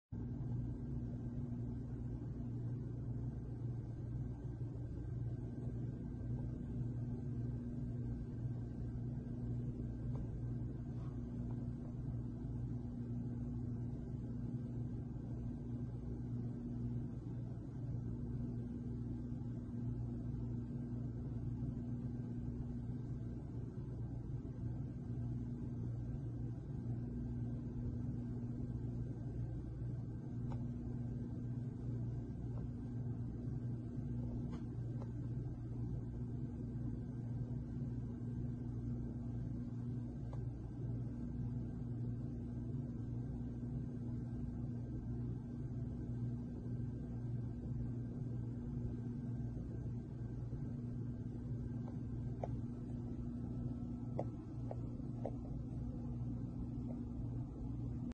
Обычный день в США
Парень остановился заправить авто и в этот момент преступники решили забрать его автомобиль.